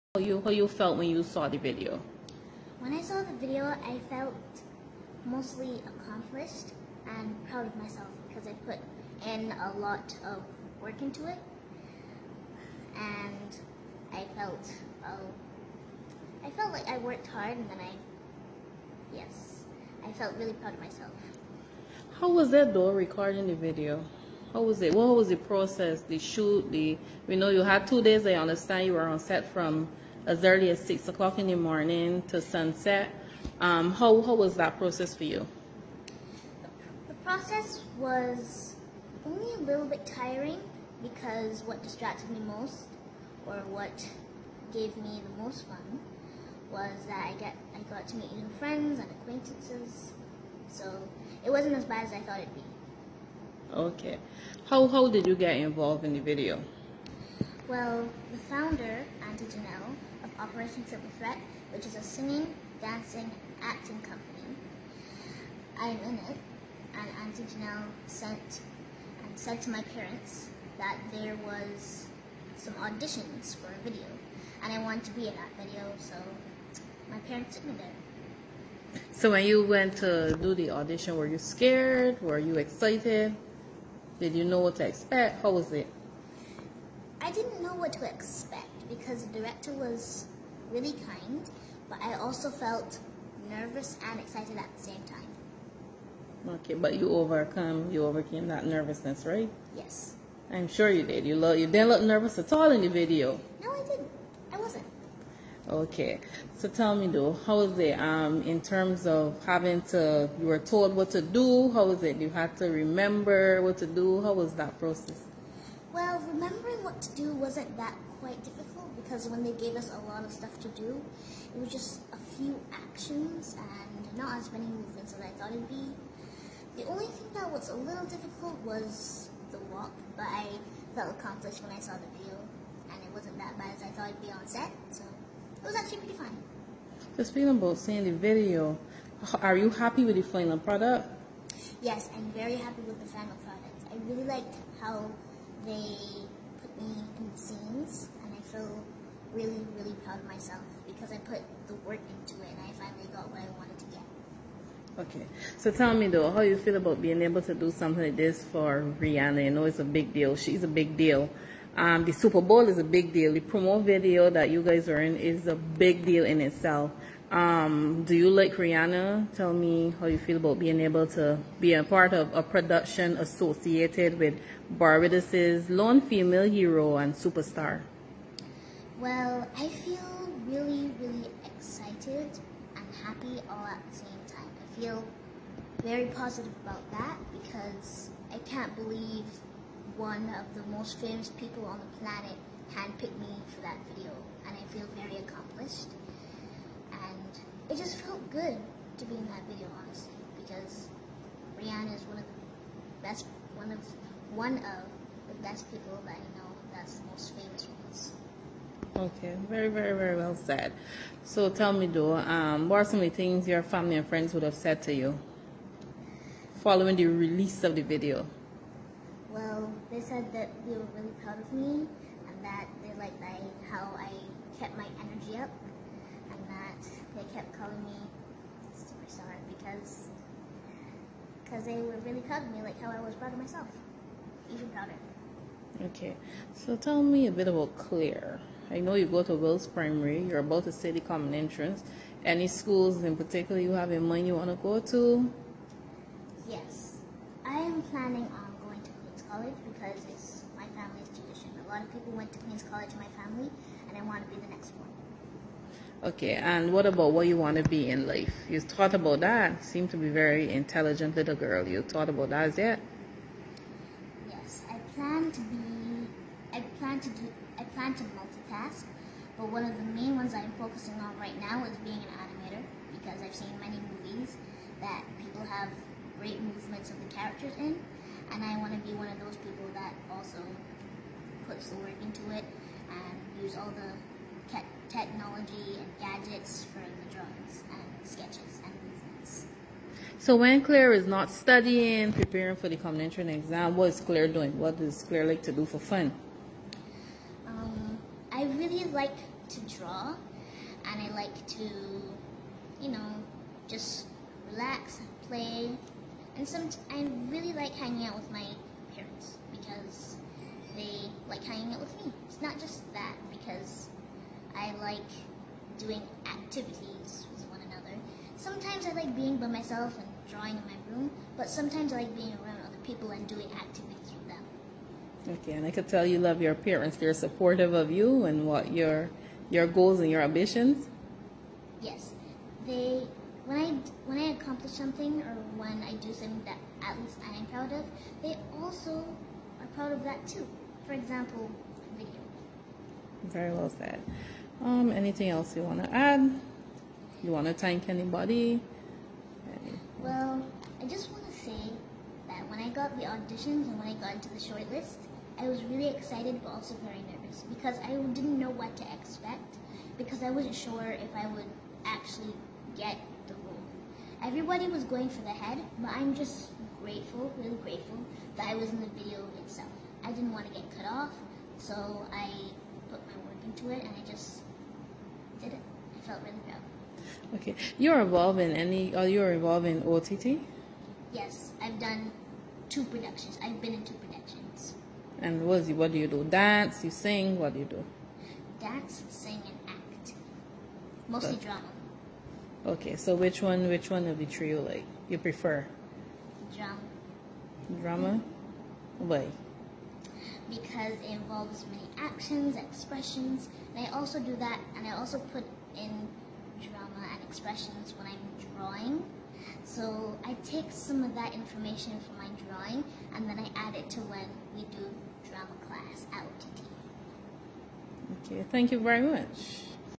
Claire proud of her role in the recent Rihanna video
Claire LaCorbiniere is a happy 10-year-old who is feeling accomplished after playing a role in the promo for the Rihanna Super Bowl LVII Halftime Show.
The Class 4 student of Wills Primary School told Barbados TODAY during an interview at her Lower Estate, St George home that she was still in disbelief that she was chosen to be a part of the 90-second video, filmed ahead of the February 12 show.
Video by BT’s Anesta Henry